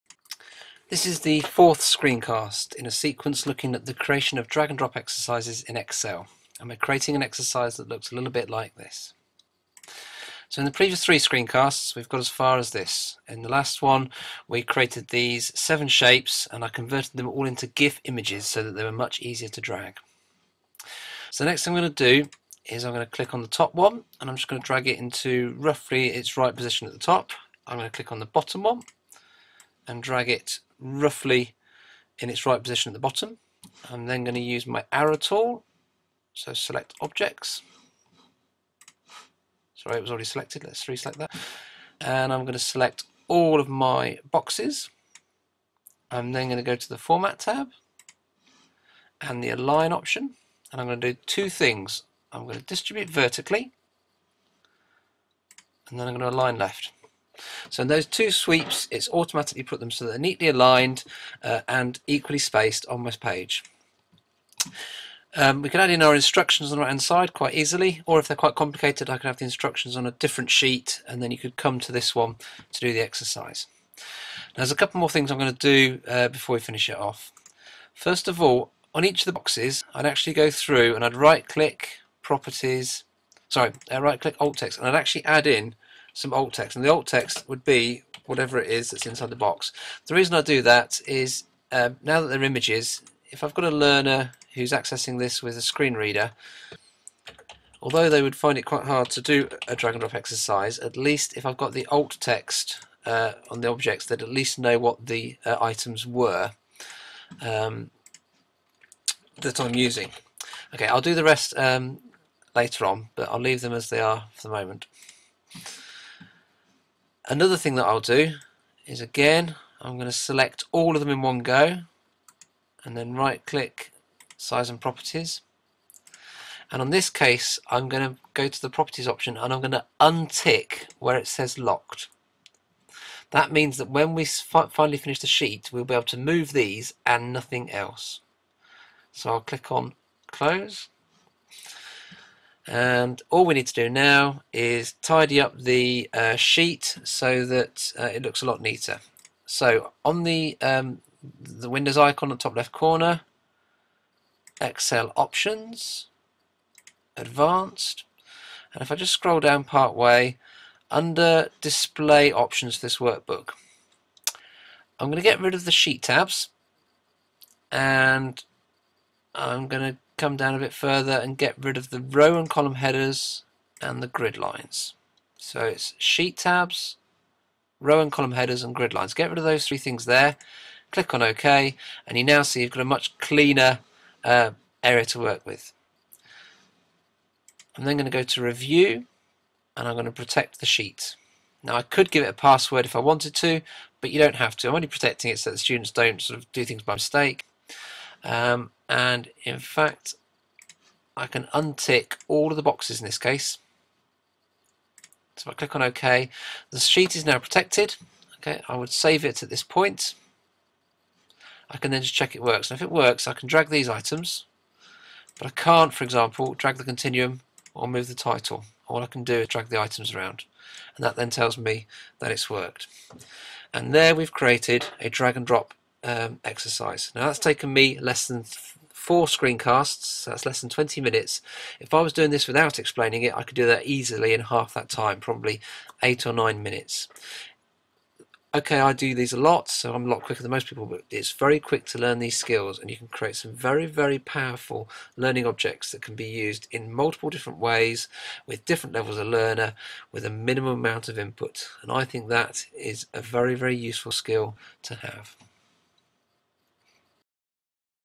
Screencast - Creating a drag & drop exercise in Excel; step 4 - Finishing it off
Screencast - Creating a drag & drop exercise in Excel; step 4 - Finishing it off. Uses skills
Align and distribute
Protecting items
ALT text
Hide gridlines